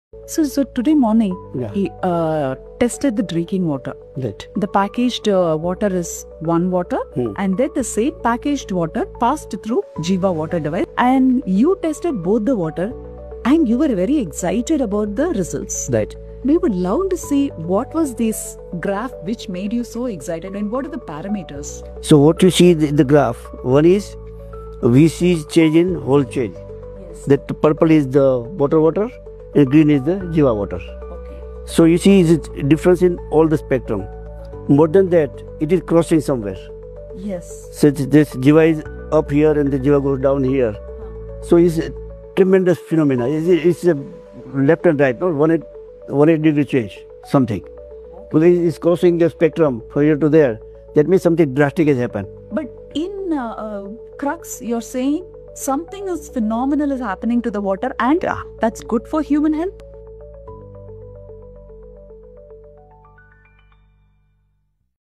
Packaged Water vs Jiva Water - Tested by a former ISRO Scientist
When a former ISRO scientist gets excited after testing Jiva Water… you know something phenomenal is happening.

Watch Dr. Madhukant Patel, founder of Reve Soils and creator of SoilX, analyze the difference between packaged water and Jiva Water. Let’s just say… the results were surprising!